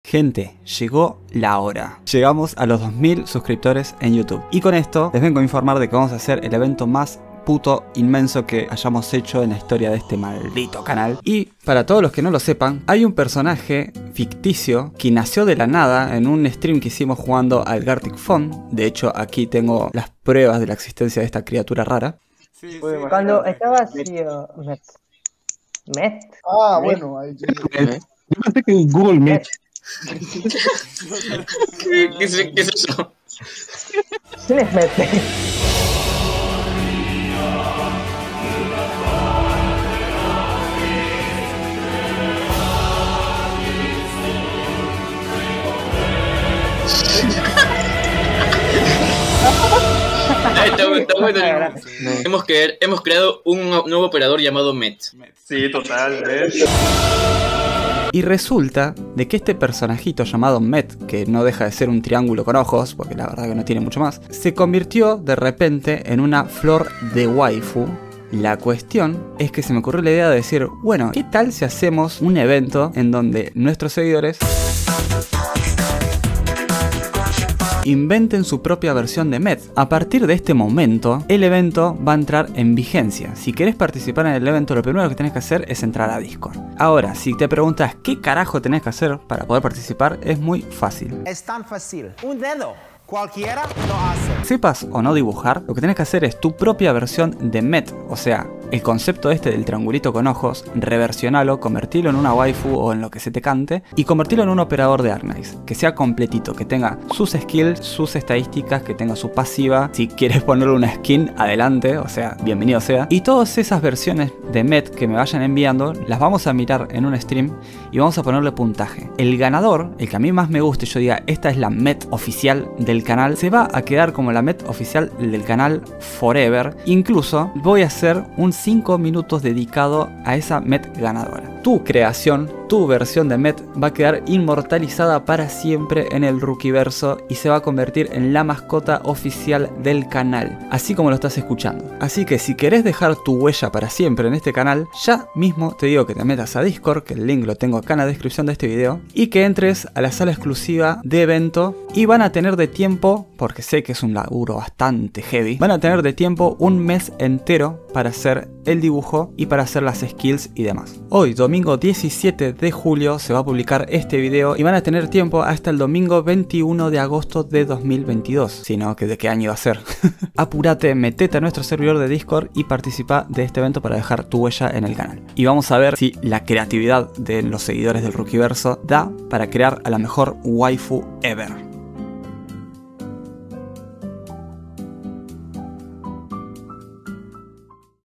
CREA TU PROPIO OPERADOR DE ARKNIGHTS!!! Evento especial, creando a Met!! || Especial 2k
PARA PARTICIPAR SUMATE A NUESTRO DISCORD!

BIENVENIDO! Eso me ayudaría muchísimo a seguir creciendo y darme motivación para crear más videos n.n